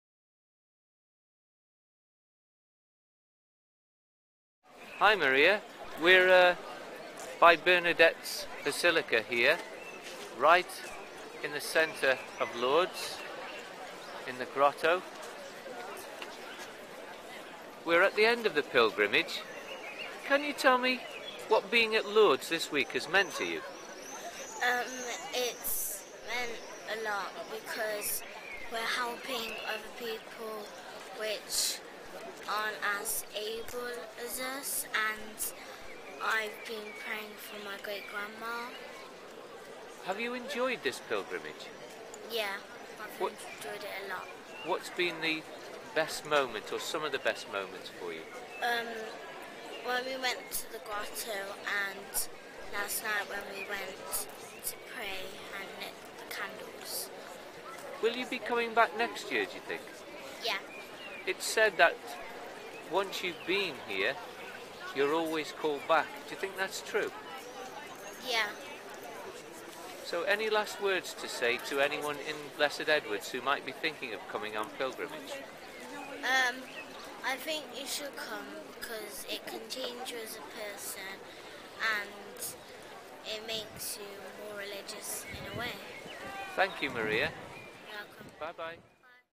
Blessed Edward Oldcorne School- Maria in Lourdes
Maria is interviewed about her first time to Lourdes as the week draws to a close.